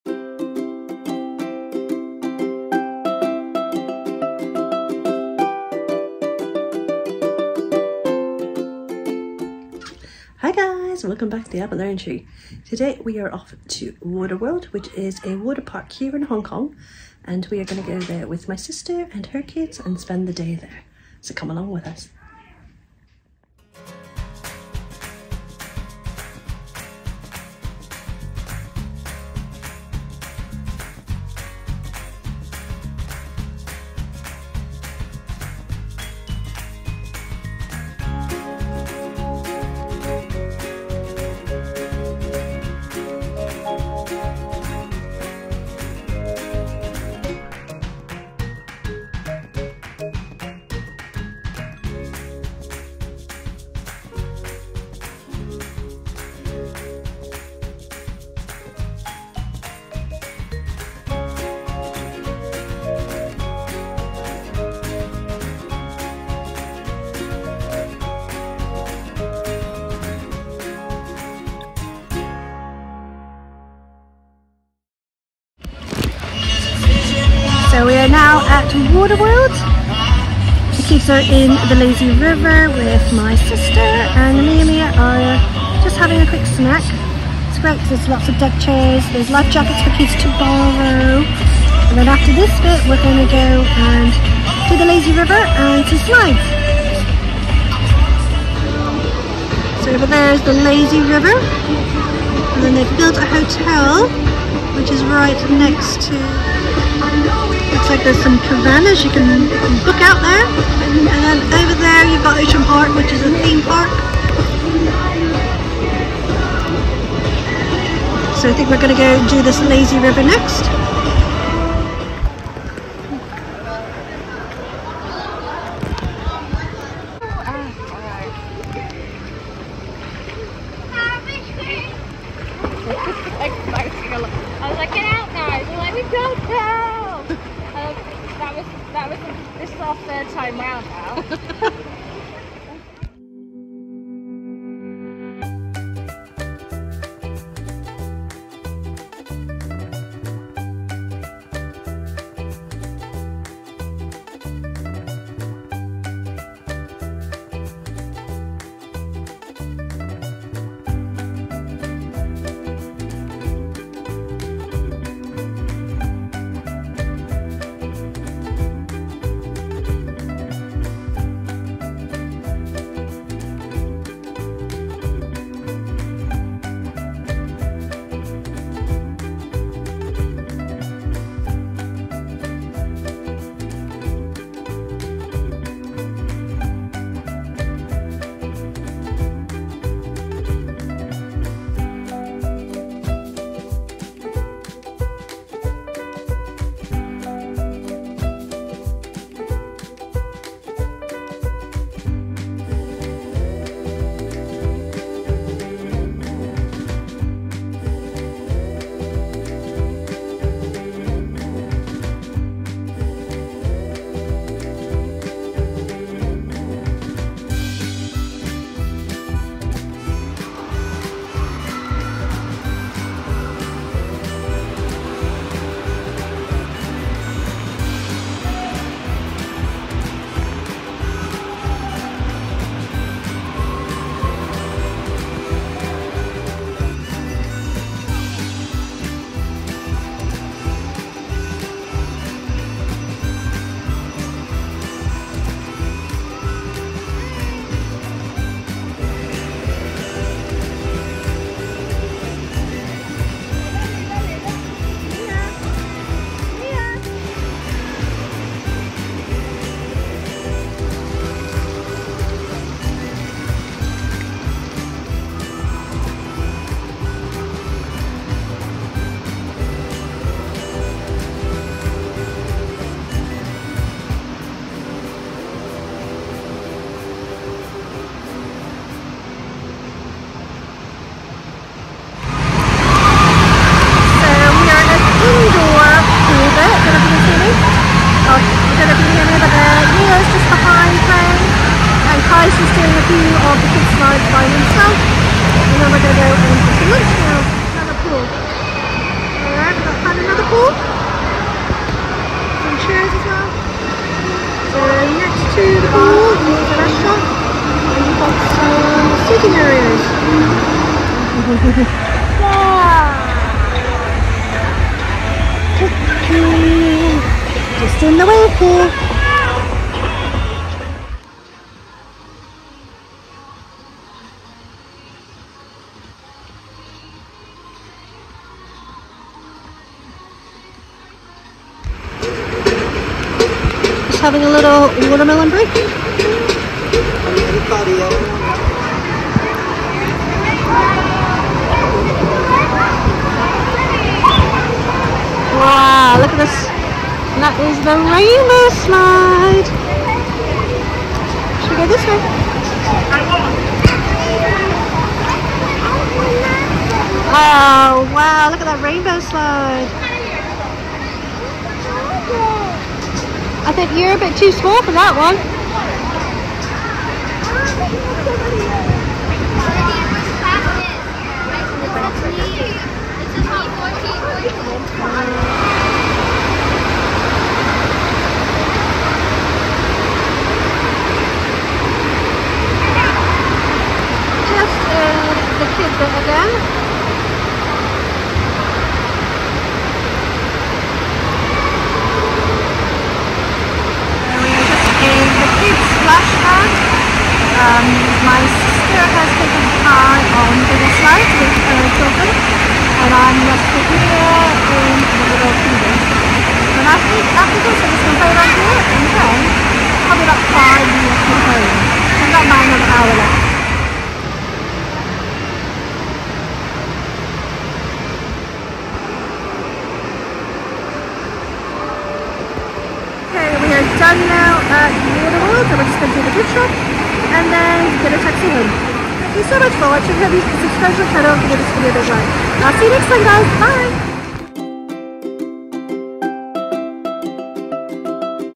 Water World Hong Kong 🇭🇰 | Travel Vlog | Theme Park
In this vlog we went to Water World which is a water theme park in Hong Kong. There are lots of different areas to explore and something for all ages! It was a great day out and definitely recommend if you’re visiting Hong Kong!